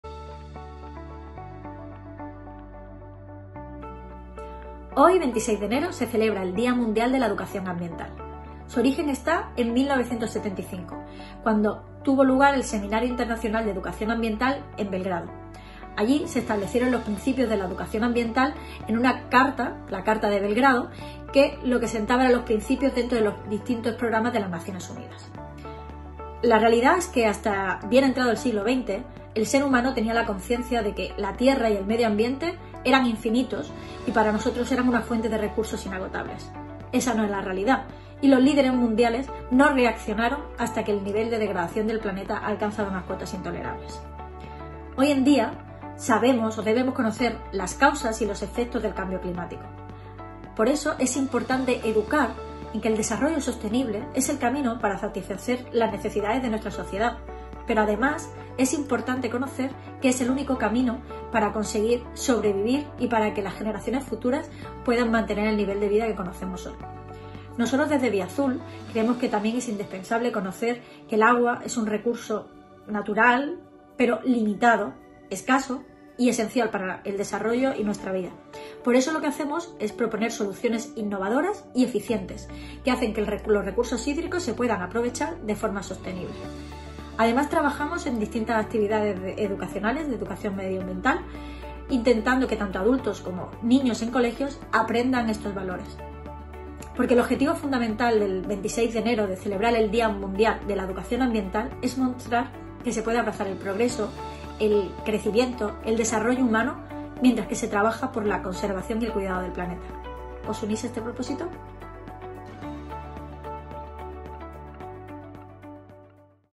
¿Por qué celebramos el Día de la Educación Ambiental?
Con motivo del DiaMundialDeLaEducaciónAmbiental nuestra compañera Ángela nos explica cuáles son las razones por las que hay que apostar por la educación ambiental y cuál es el papel del agua.